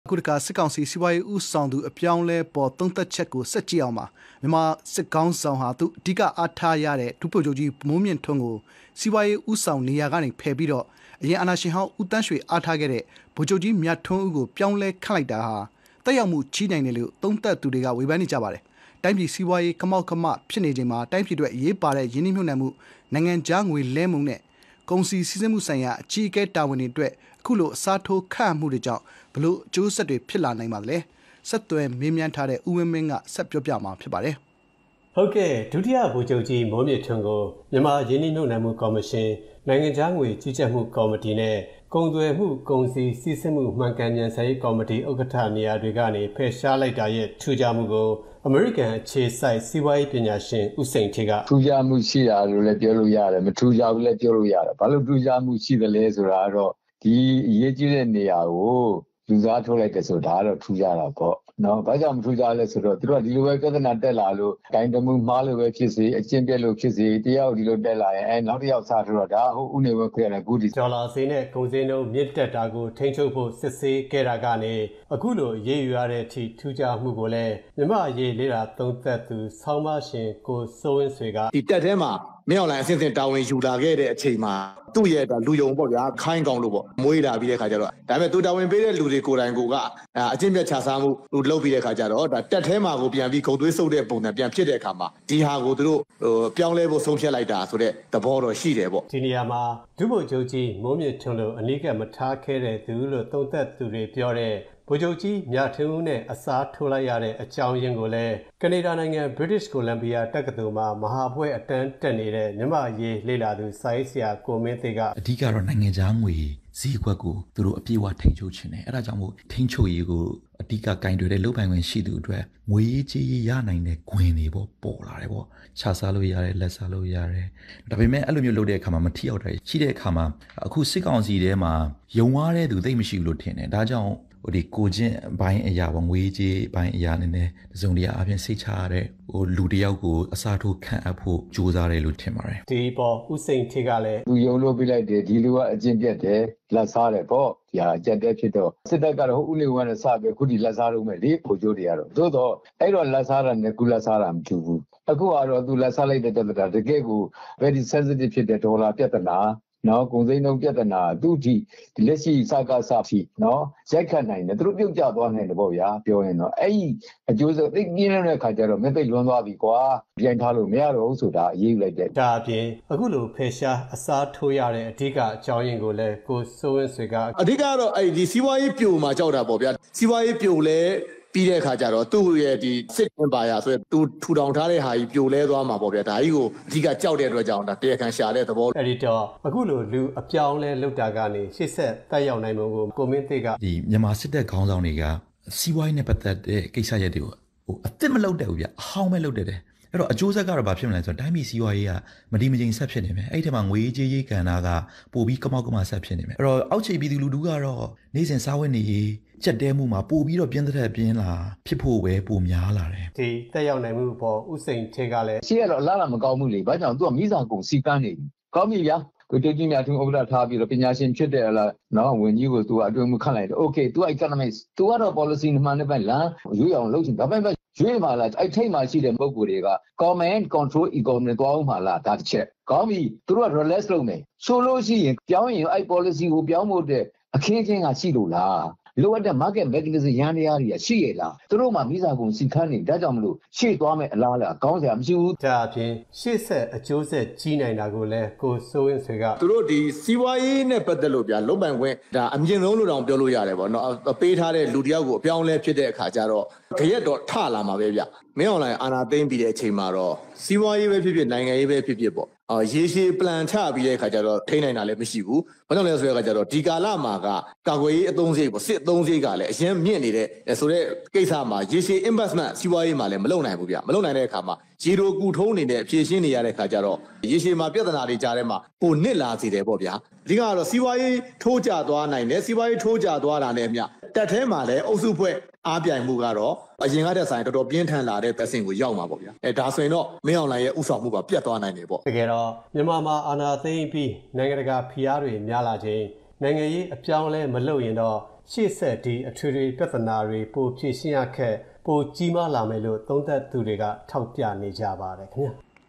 စစ်ကောင်စီ စီးပွါးရေးဦးဆောင်သူ အပြောင်းအလဲအပေါ် အမြင်တချို့
မြန်မာစစ်ကောင်စီ ခေါင်းဆောင်ဟာ သူအဓိကအားထားခဲ့တဲ့ ဒုဗိုလ်ချုပ်ကြီး မိုးမြင့်ထွန်းကို စီးပွါးရေးဦးဆောင် နေရာကနေဖယ်ရှားပြီး အရင်အာဏာရှင်ဟောင်း ဦးသန်းရွှေအားထားခဲ့တဲ့ ဗိုလ်ချုပ်ကြီး မြထွန်းဦးကို ပြောင်းလဲခန့်အပ်လိုက်တာဟာ သက်ရောက်မှု ကြီးနိုင်တယ်လို့ သုံးသပ်သူတွေက ဝေဖန်ပါတယ်။  ကိုဝင်းမင်း ပြောပြမှာပါ။